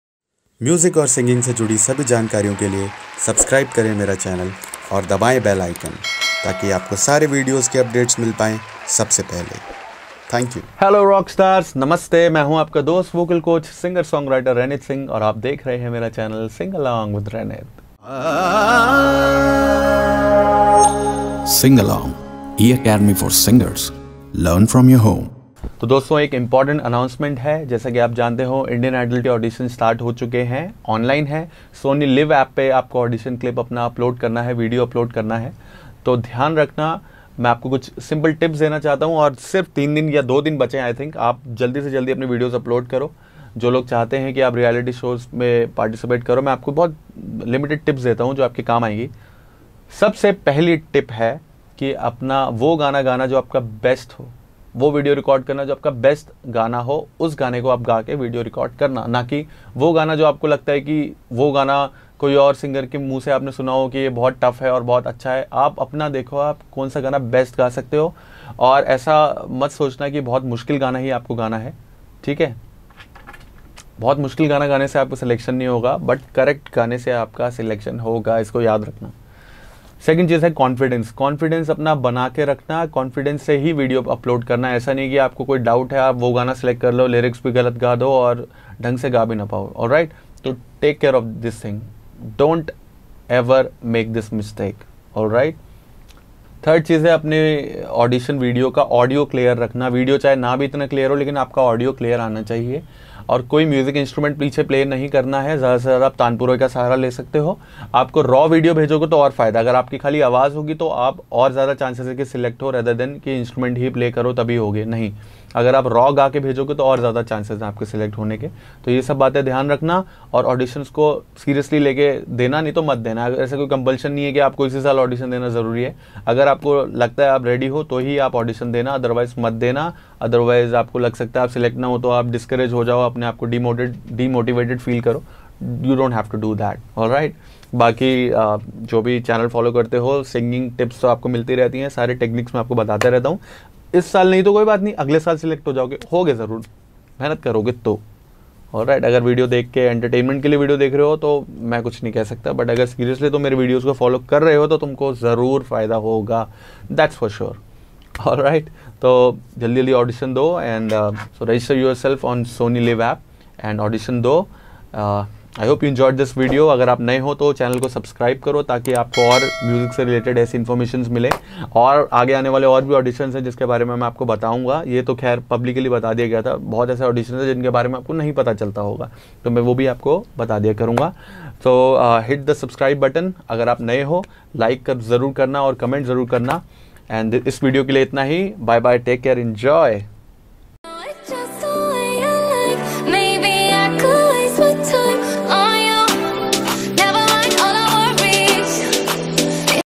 Audition Tips | Indian Idol Audition | Advice by Reality show Contestant | Episode - 74 | Sing Along
Raenit Singh ( Singer/Song Writer/Vocal Coach )